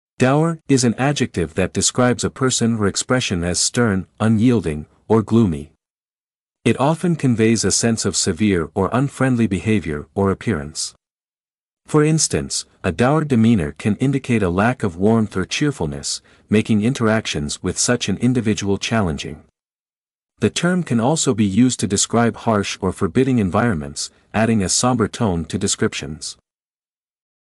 What is the meaning of Dour?
In this video, we explain the meaning of the word "dour." The term refers to someone or something that is stern, unyielding, or grim in manner or appearance. It can describe a person who is serious and gloomy or a situation that feels bleak. We keep the explanation concise and to the point, focusing solely on the definition and common usage of the word "dour" in everyday language. This video is perfect for anyone looking to expand their vocabulary or gain a clearer understanding of this term.